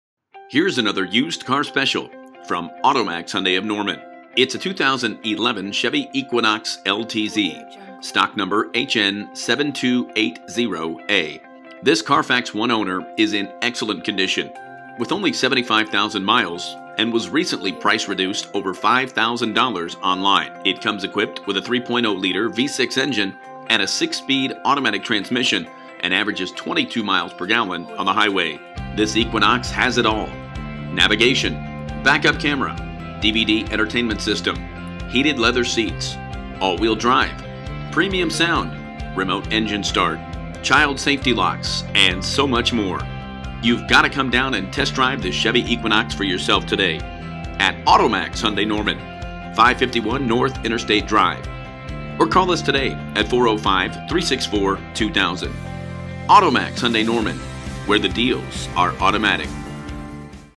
Used 2011 Chevy Equinox LTZ in Oklahoma City, Edmond, & Norman, OK
In this video we give you a one-minute review of this Used 2011 Chevy Equinox LTZ we have for sale at AutoMax Hyundai Norman. We are the #1 Hyundai dealer in Oklahoma City, Edmond and Norman, OK.